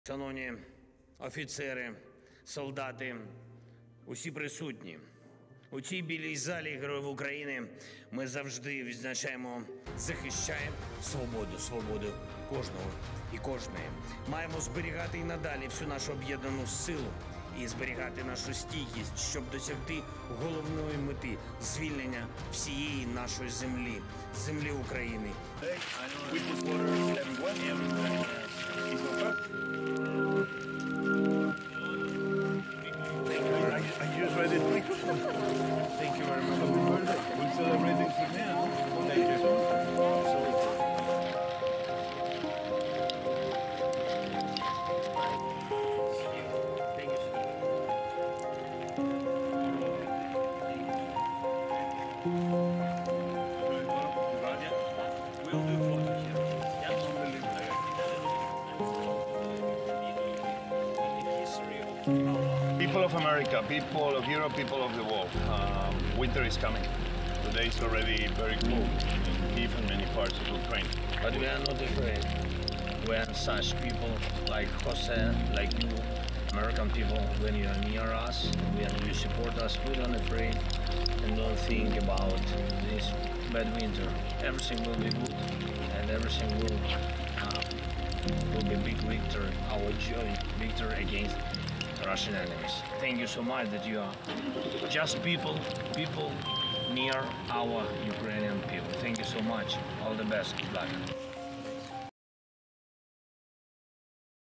Zelenski premia al chef José Andrés por su ayuda y compromiso con Ucrania
El Gobierno de Ucrania ha premiado al chef José Andrés, fundador de World Central Kitchen, entre otros galardonados, por su compromiso en el Día de la Dignidad y de la Libertad en Ucrania.

Posteriormente, el cocinero y el presidente acudieron al Paseo de los Valientes donde se dedicó una placa en honor al trabajo y la ayuda que el chef y su fundación han brindado a la población ucraniana durante la guerra.